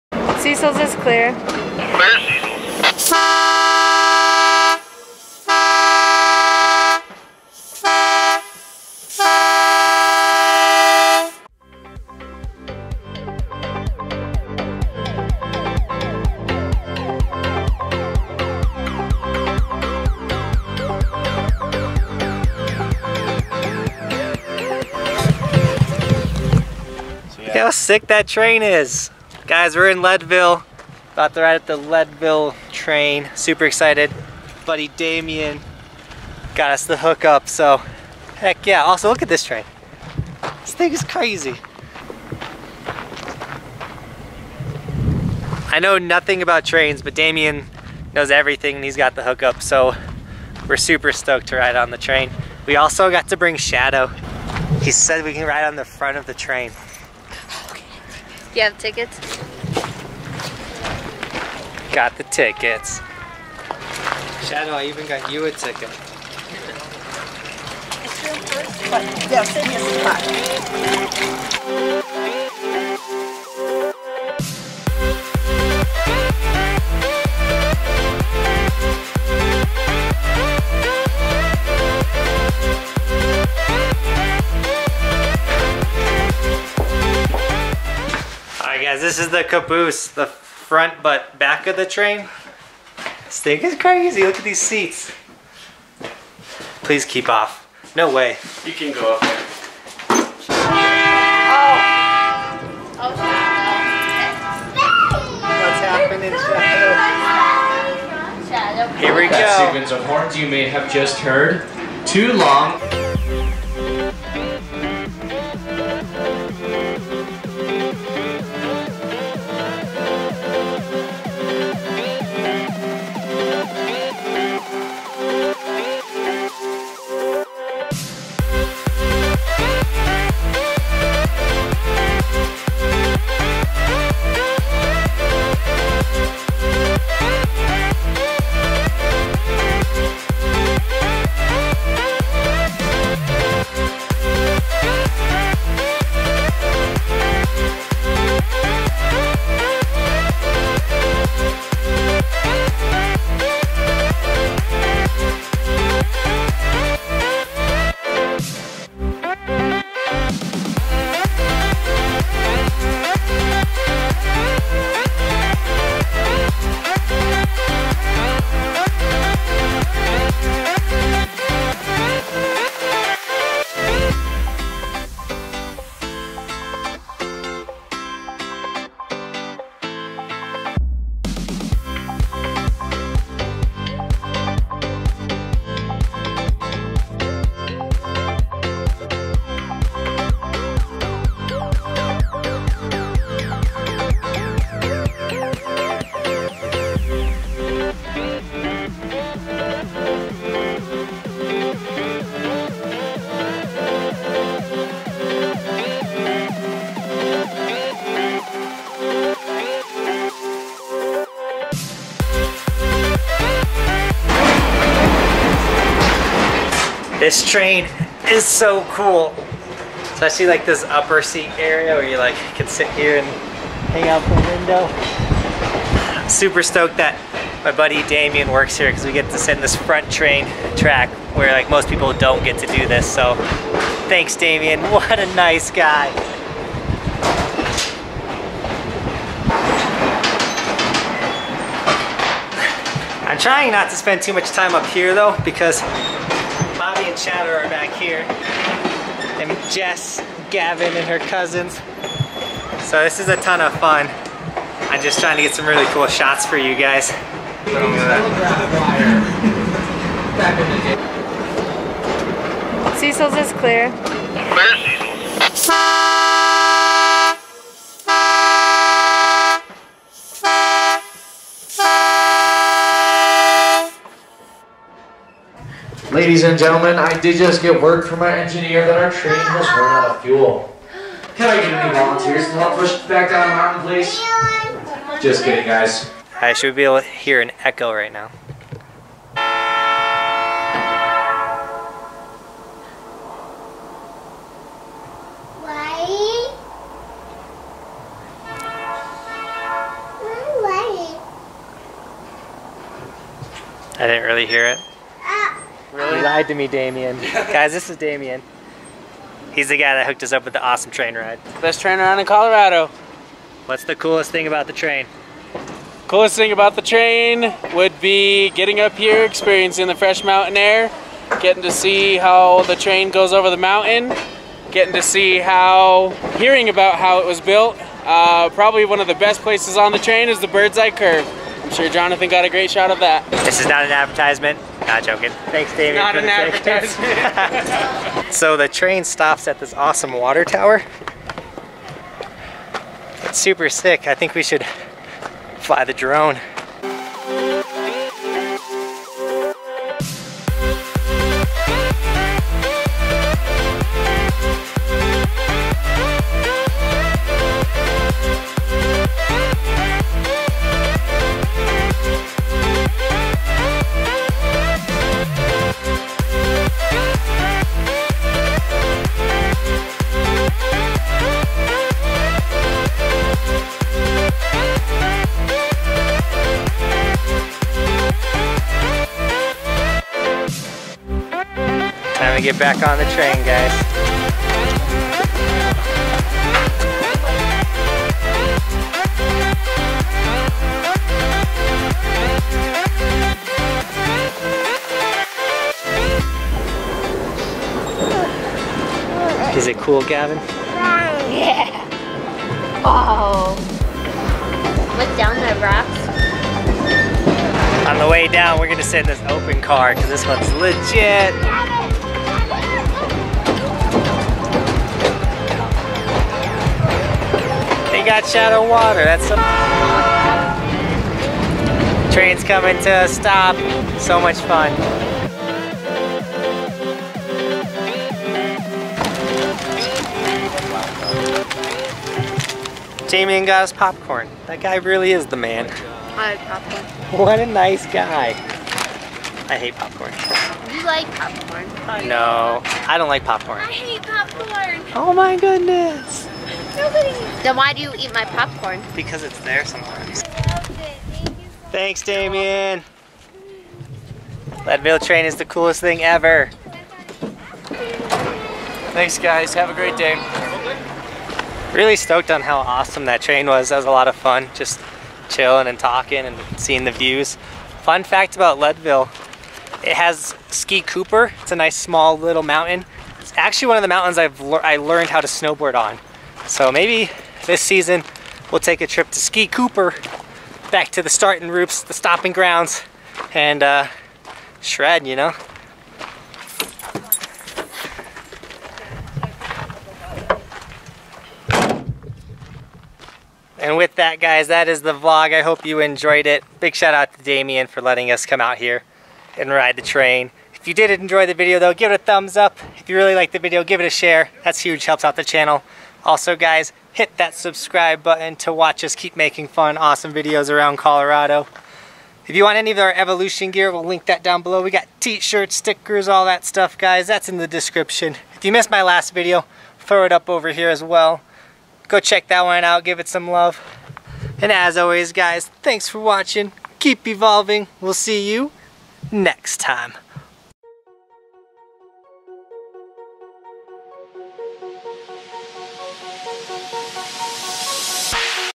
The Leadville Train - Leadville Colorado
My Buddy asked me if I wanted to take Bobbie on an awesome train ride and of course I said yes! Short Vlog but an awesome one!

Stickers!

Skateboarding Evolution Sticker

What a Nice Guy Sticker

New Die Cut Evolution Sticker

Blog Gear!

Social media!